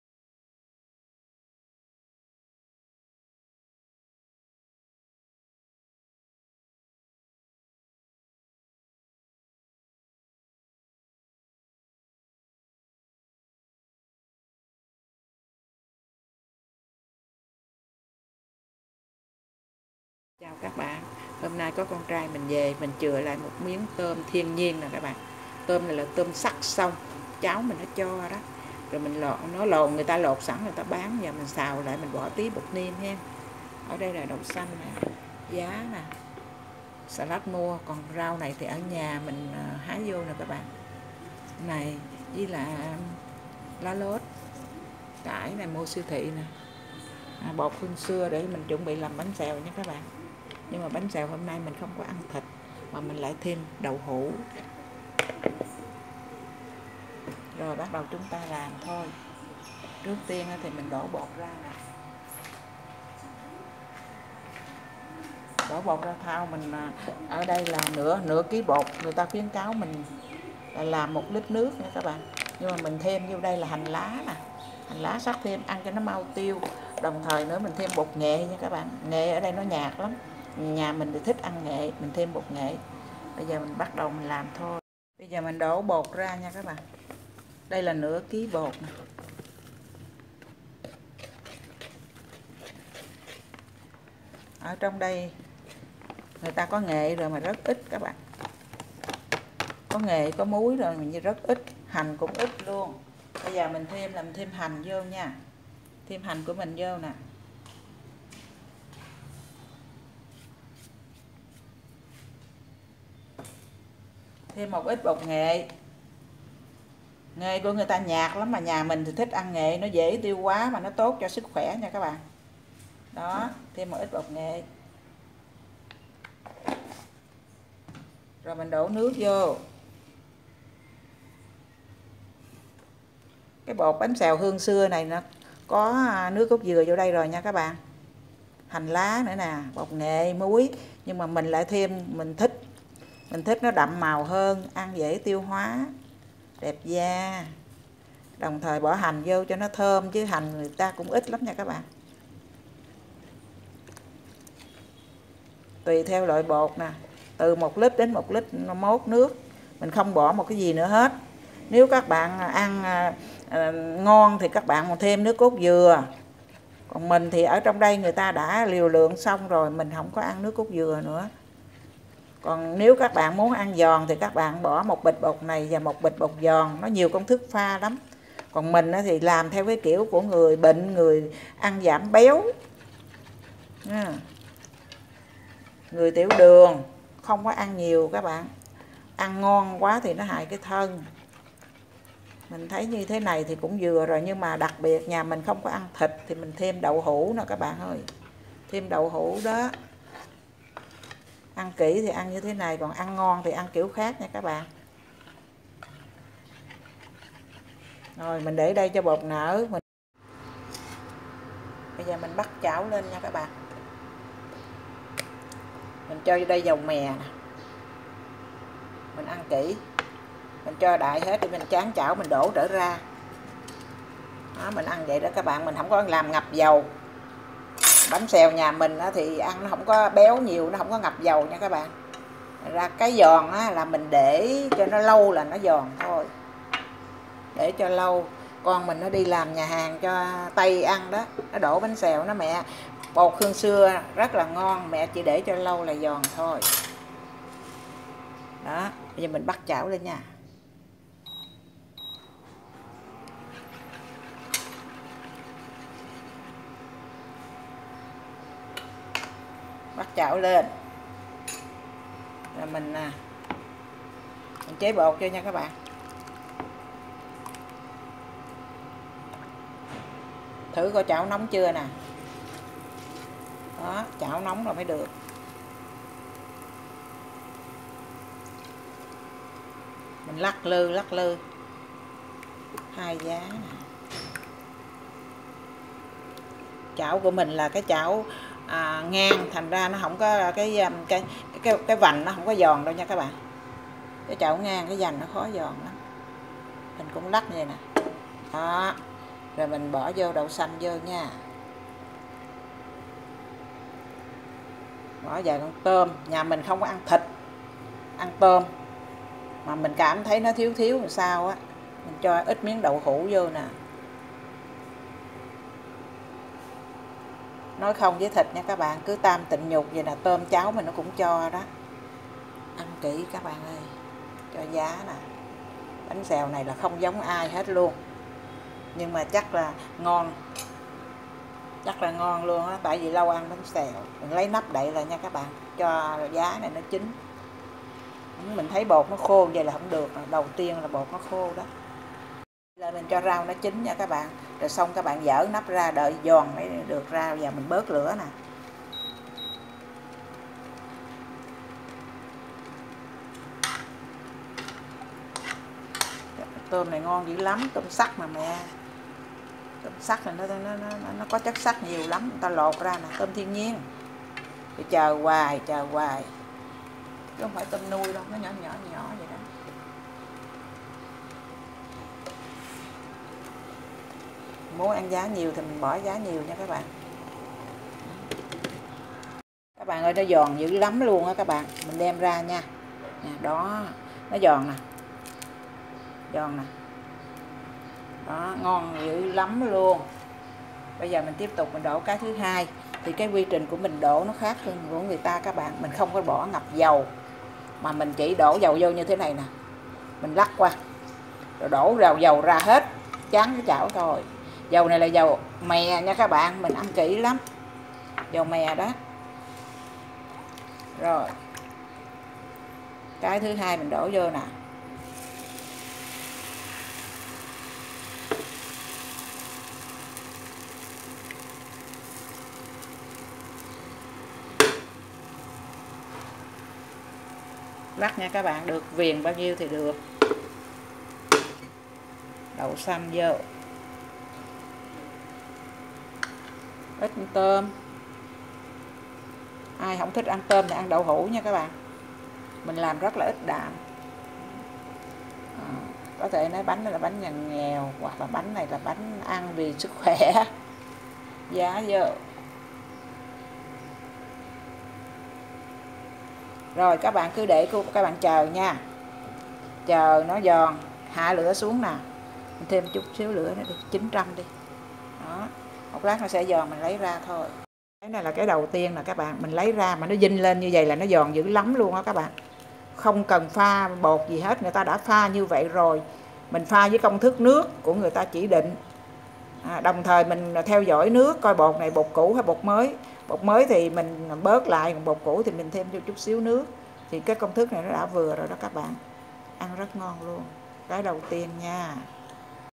Hôm nay mẹ làm bánh xèo sao quá đơn giản .Giòn rụm cả ngày ..ai cũng khen ...@Khỏe Trần
Bột pha sẵn hương xưa. .500g .
1l nước lọc..
thêm hành và nghệ .